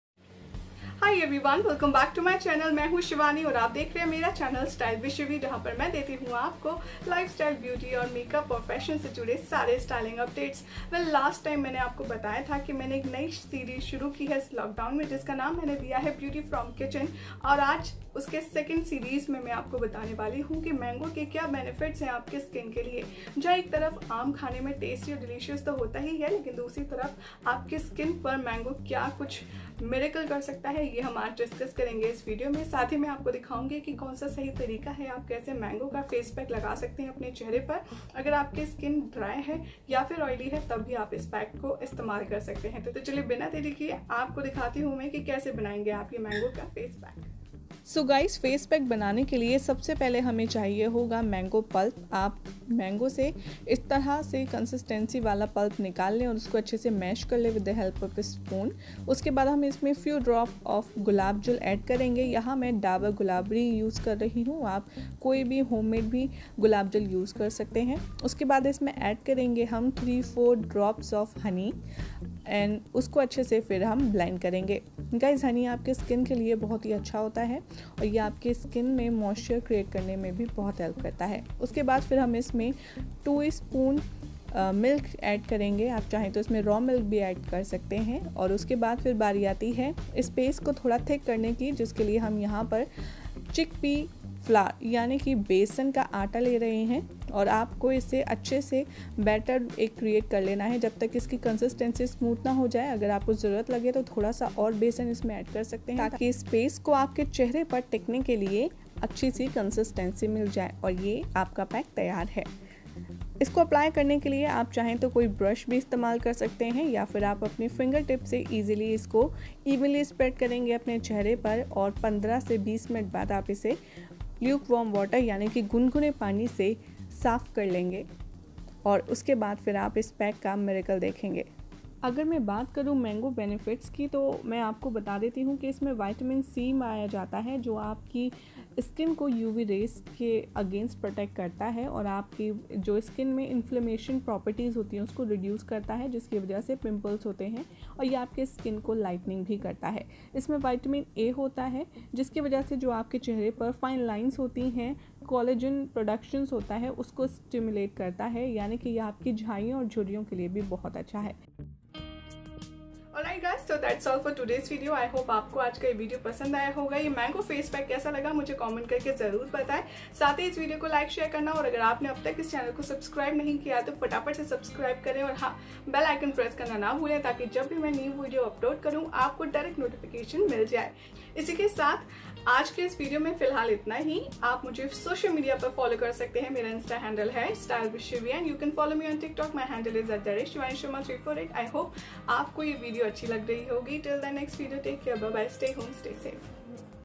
Mango Face Mask for Milky Whiten Skin । Mango Facial at Home । Summer Fruit Facial । stylewithshivi
*Content Cover*

2min Glowing pack for dry and dull skin. Mango benefits for your skin . Hope you will like it